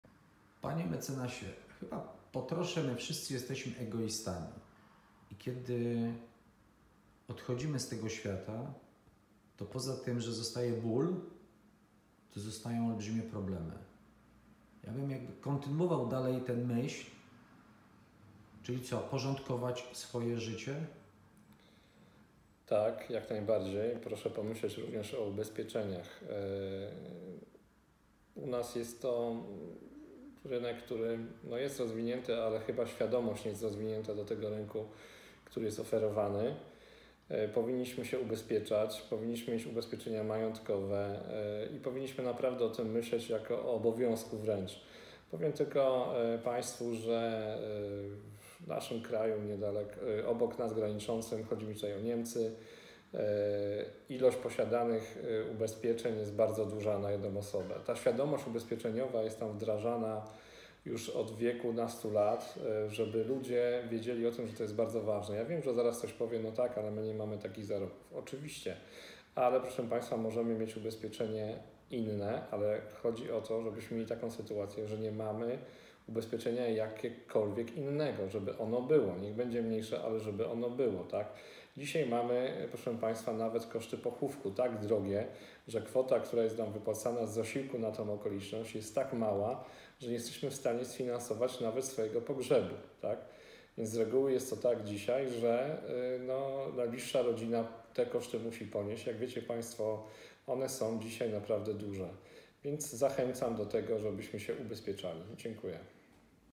Porady Prawne. Radca prawny Rafał Krupiński. Dbajmy o bezpieczeństwo, korzystajmy z ubezpieczeń. #10
Radca prawny Rafał Krupiński świadczy bezpłatnie pomoc prawną na terenie parafii pw. św. Klemensa Hofbauera w Głogowie. Dbajmy o bezpieczeństwo swoje i rodziny, korzystajmy z ubezpieczeń.